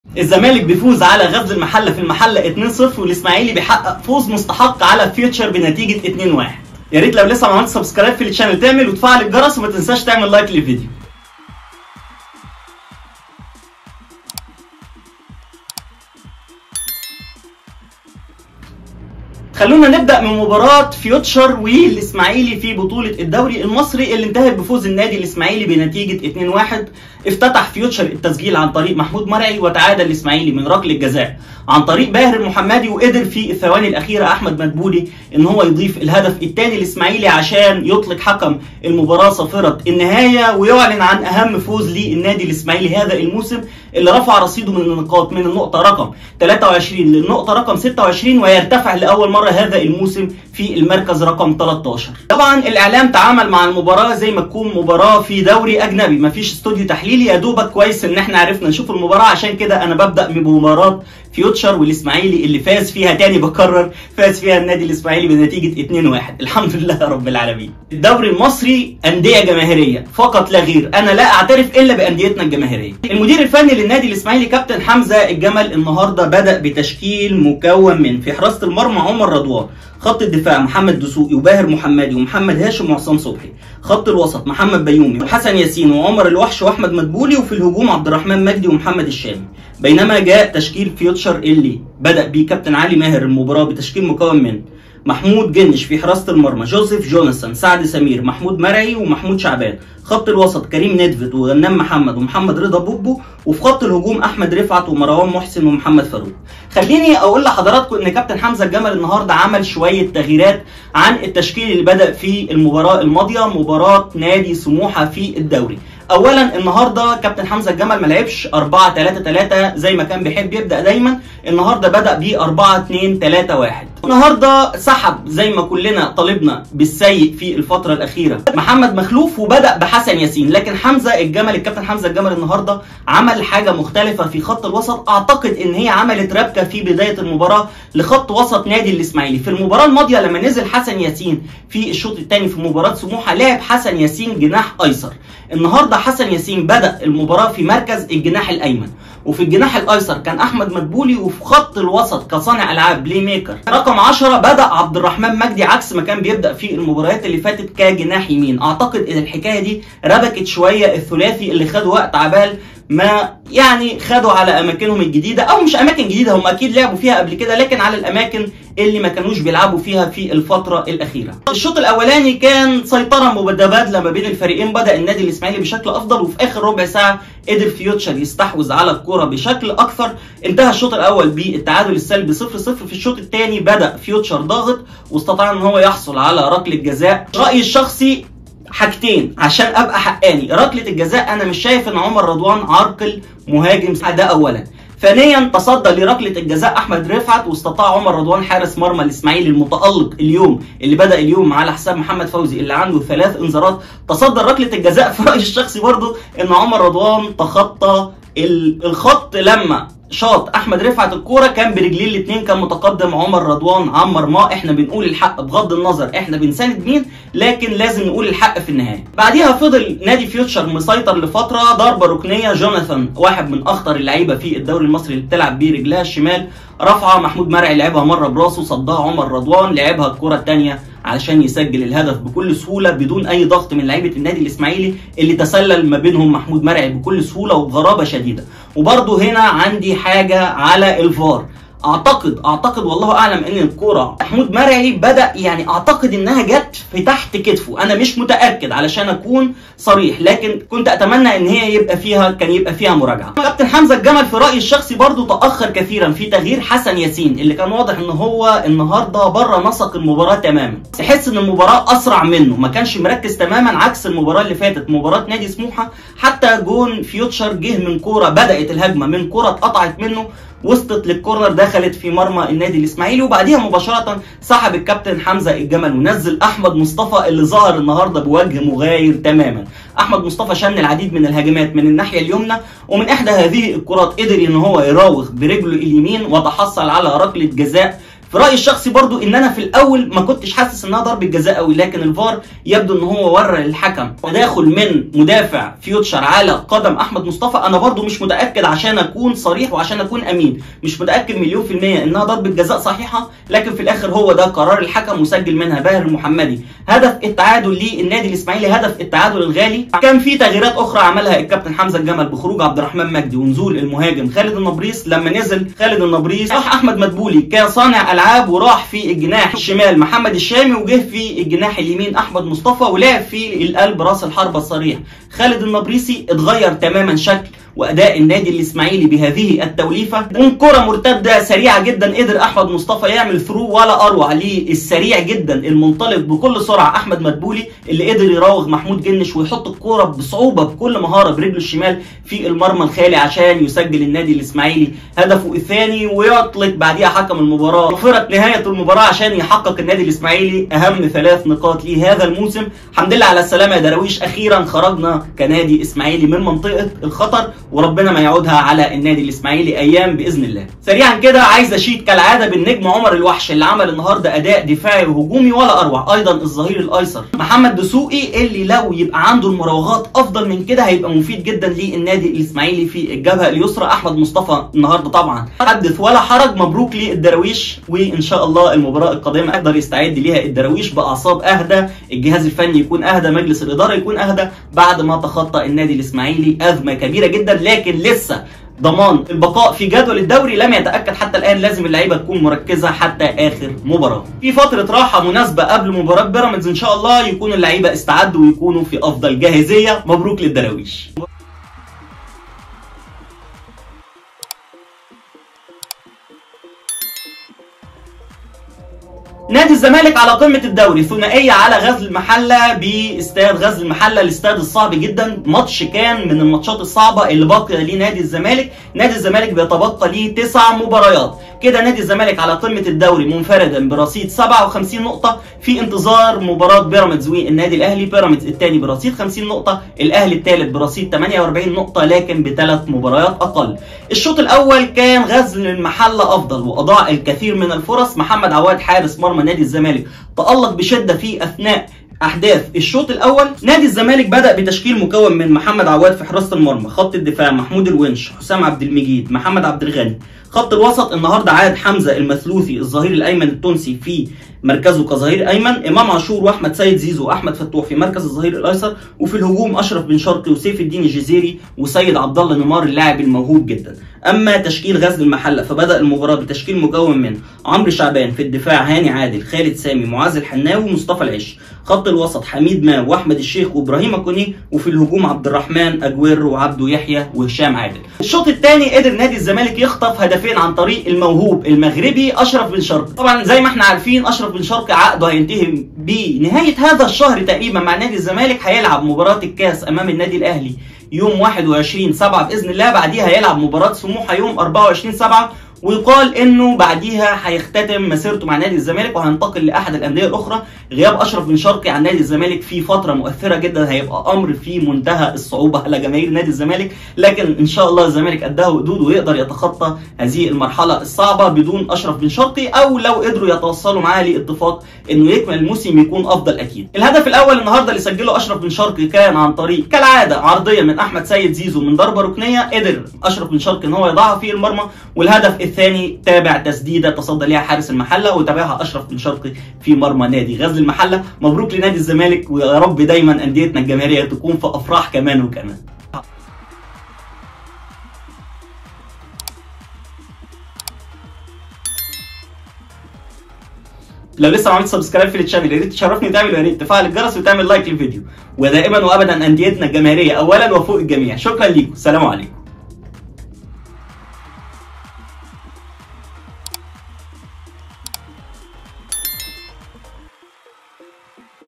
الاسماعيلي يفوز علي فيوتشر ٢-١ حمد لله علي السلامة يا دراويش|الزمالك علي القمة بفوز علي غزل المحلة
حقق فريق الإسماعيلي فوزا مثيرا على فيوتشر بهدفين مقابل هدف وحيد فى المباراة التي جمعت الفريقين مساء اليوم، السبت، باستاد السلام، ضمن منافسات الجولة 25 من عمر مسابقة الدوري المصري الممتاز.

تقدم فريق فيوتشر فى الدقيقة 62 من عمر المباراة، وجاء الهدف من ركلة ركنية لصالح فريق فيوتشر من جهة اليمين نفذها جوزيف نجويم عن طريق كرة عرضية إلى داخل منطقة الجزاء قابلها محمود مرعي برأسية أبعدها عمر رضوان عن مرماه ثم عادت لمرعي مرة أخرى فسدد كرة سكنت يسار المرمى.

وأدرك باهر المحمدي مدافع الإسماعيلي التعادل لفريقه أمام فيوتشر بالدقيقة 77 فى المباراة، وجاء هدف التعادل من ركلة جزاء تحصل عليها الإسماعيلي واحتسبها الحكم طارق مجدي بعد الرجوع لتقنية الفار، قبل أن ينجح أحمد مدبولى في تسجيل هدف قاتل في الدقيقة الثالثة من الوقت المحتسب بدلا من الضائع.

بهذه النتيجة يحتل فيوتشر المركز الرابع فى جدول مسابقة الدوري العام برصيد 40 نقطة، بعدما خاض 25 مباراة، فاز فى 11 مباراة وتعادل فى 6 لقاءات وخسر 8 مباريات أخرى، وسجل لاعبو فيوتشر 37 هدفا واستقبلت شباكهم 27 هدفا، فيما ارتقى الدراويش للمركز 13عشر بعد خوض 24 لقاء، حيث تعادل في 8 مباريات وخسر 10 مواجهات، وسجل لاعبوه 20 هدفاً وتلقت شباكهم 27 هدفاً.

وحقق الزمالك فوزا مستحقا على غزل المحلة بهدفين نظيفين في المباراة التى جمعتهما مساء اليوم، باستاد غزل المحلة، ضمن منافسات الجولة 25 من عمر الدوري. وبهذه النتيجة وصل الزمالك للنقطة 57 فى صدارة ترتيب جدول الدوري المصري.

سجل ثنائية الزمالك فى شباك المحلة أشرف بن شرقي، وشهدت المباراة تألق محمد عواد الذى دافع عن مرماه فى أكثر من فرصة محققة لأصحاب الأرض.

نجح فريق الزمالك في تحقيق الفوز على نظيره غزل المحلة بهدفين نظيفين في المباراة التي جمعتهما اليوم ضمن مباريات الجولة الـ 25 للدورى ، ليستمر الزمالك في صدارة جدول الترتيب بعد خوضه 25 مباراة جمع خلالها 57  نقطة بعدما حقق الفوز في 18 مباراة، وتعادل في 3 مباريات وخسر 4 مباريات وسجل لاعبوه 44 هدفاً وتلقت شباكه 25، فيما تجمد رصيد المحلة عند 26 نقطة جمعها بالفوز في 5 مباريات وتعادل في 11 مباراة وخسر 9 مباريات وسجل لاعبوه 21 هدفاً وتلقت شباكه 30 .